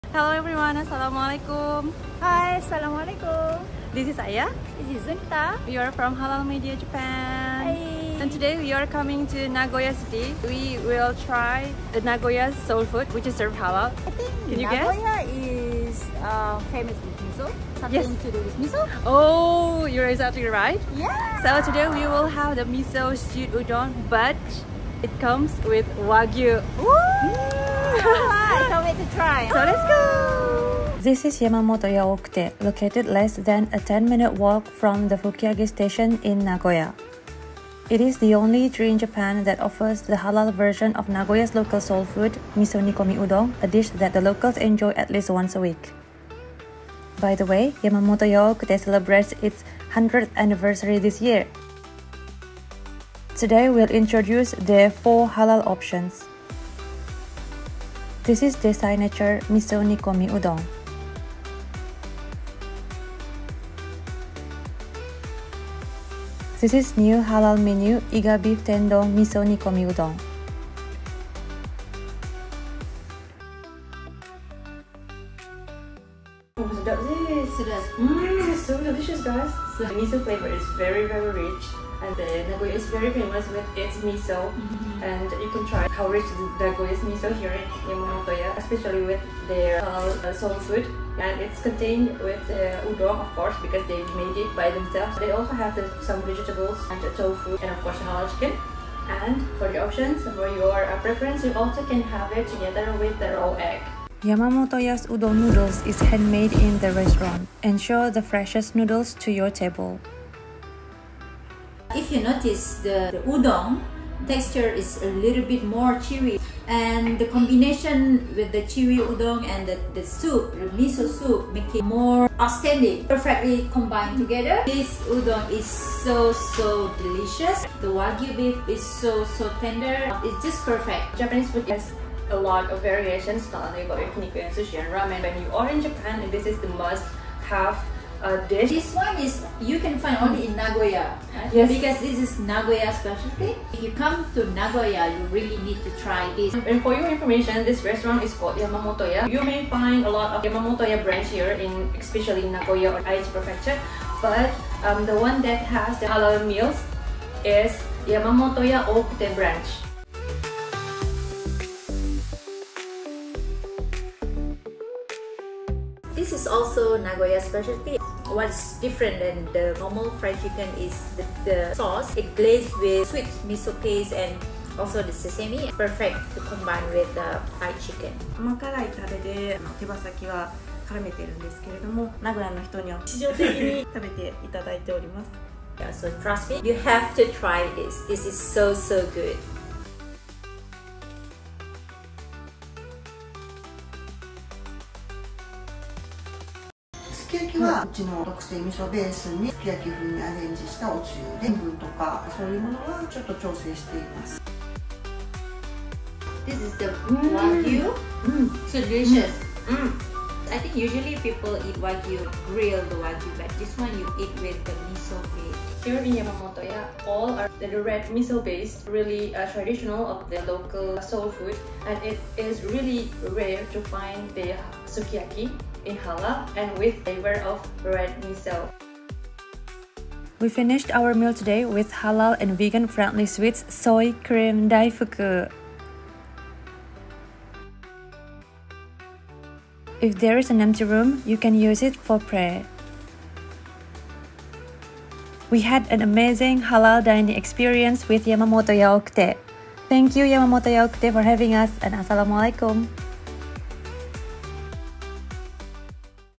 Discover Halal Nagoya Soul Food: Authentic Flavors in the Heart of Nagoya!
Since 2018, Yamamotoya Ookute has delighted customers with its Halal miso nikomi udon, a cherished delicacy in Nagoya. This renowned restaurant has become a favorite among Muslim visitors due to the limited Halal dining options in the city.

Yamamotoya Ookute continues to innovate and expand its menu. In May 2024, the restaurant proudly introduced two new culinary delights: Halal Wagyu Sukiyaki and Iga Beef Tendon Miso Nikomi Udon.